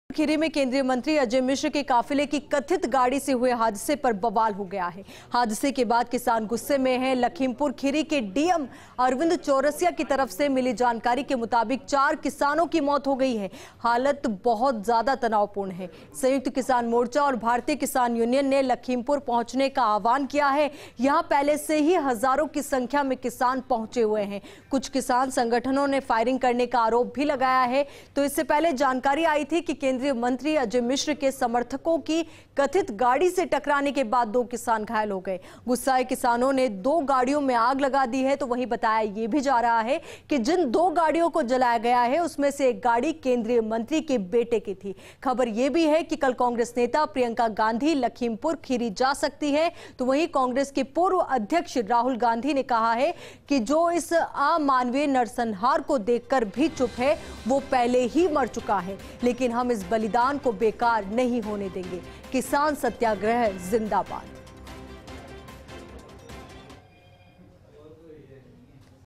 UP में कथित BJP नेता की गाड़ी से 4 किसानों की मौत के बाद स्थिति तनावपूर्ण
About this Video:
Situation tense in UP after alleged BJP leader's car killed 4 farmers